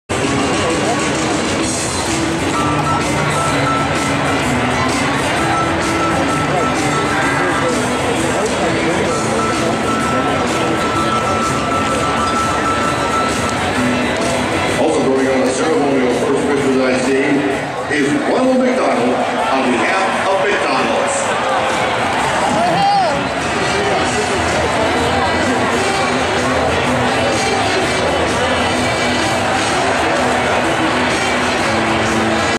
Ronald as a pitcher. Not as good as he thinks!
US Cellular Field.  August 2012 Sox vs. Angels.  Chewing gum and getting ready for the White Sox Game in the right field corner.   Comments turned off by YouTube!